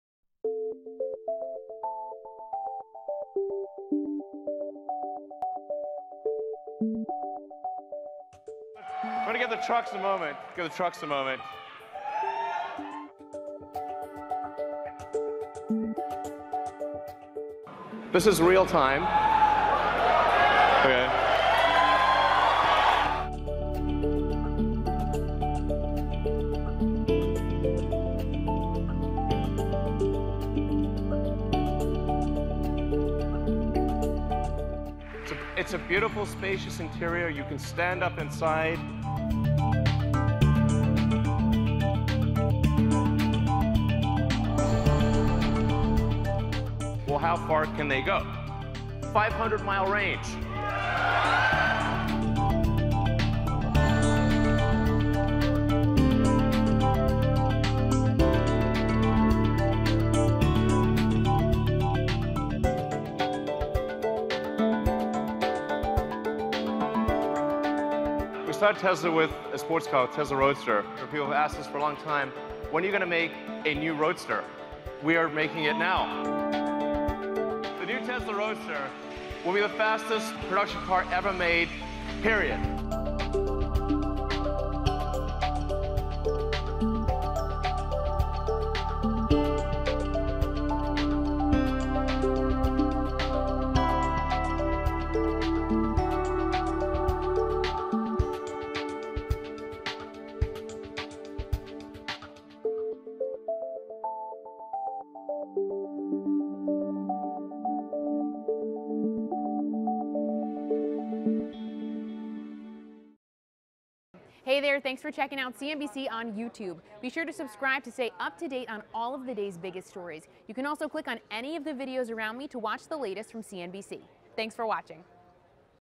Tesla Just Threw In A Major Surprise At The Semi Truck Unveiling A Revamped Roadster CNBC
Tesla's Semi, an electric truck that delvers high speed, high power